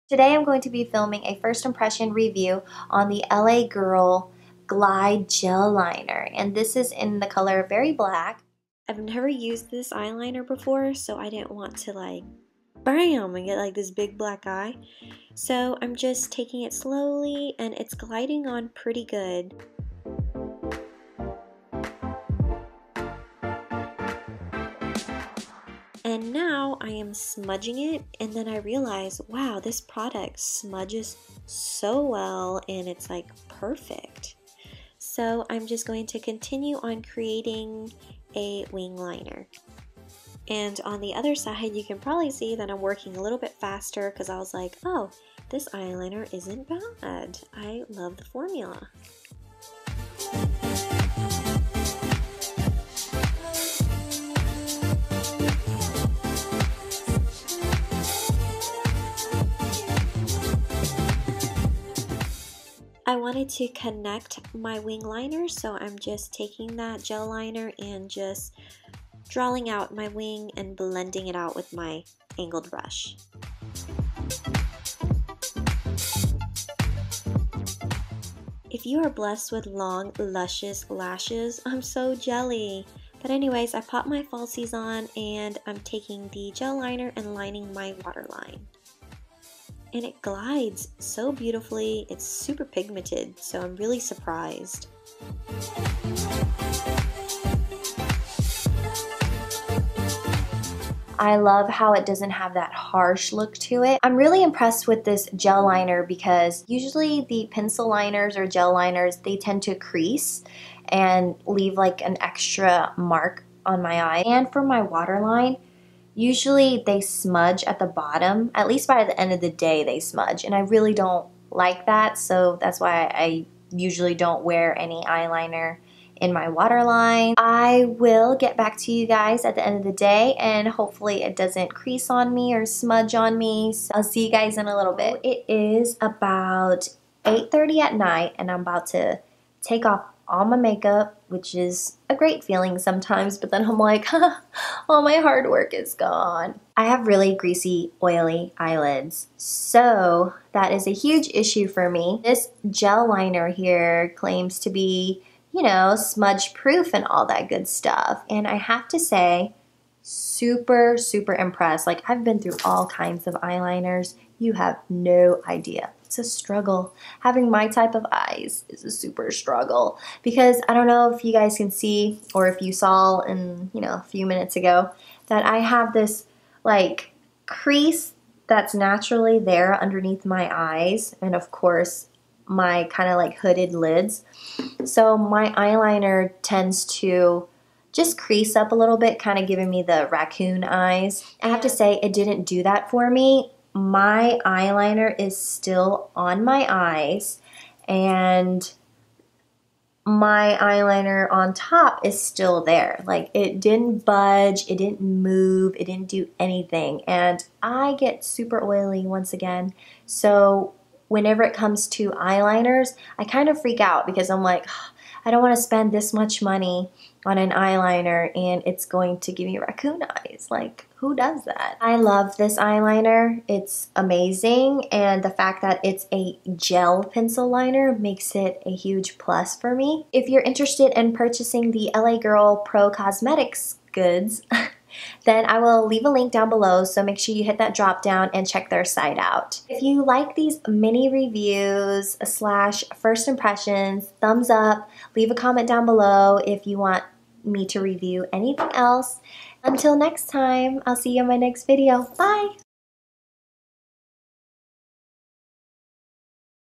L.A. Girl Pro Glide Gel Liner First Impression/Review | SMUDGE PROOF
Heyyyy loves!

Hope you ENJOY my mini review/first impression!

XOXO

I DEF have progressed and grew with my videos and editing. YAY! So, it gets better from here on out loves!

Thank you L.A. Girls Cosmetics for sending me the wonderful goods!